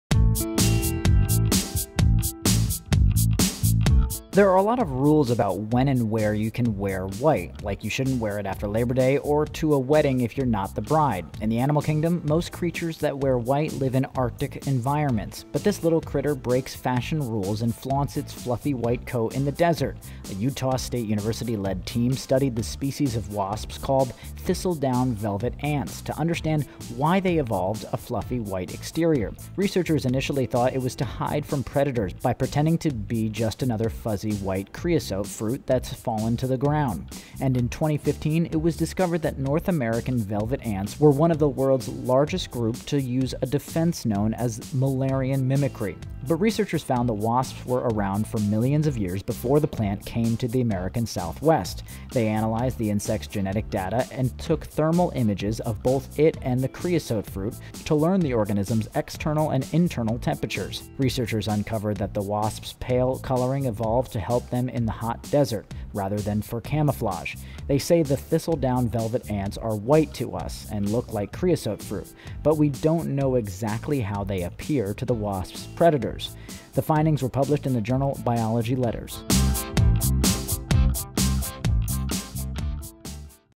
Scientists Discover How This Velvet Ant Got So Fluffy and White
The thistle-down velvet ant's name isn't the only thing that's misleading. Researchers learned "not to judge a book by its cover" while studying why these wasps evolved a white, fluffy exterior.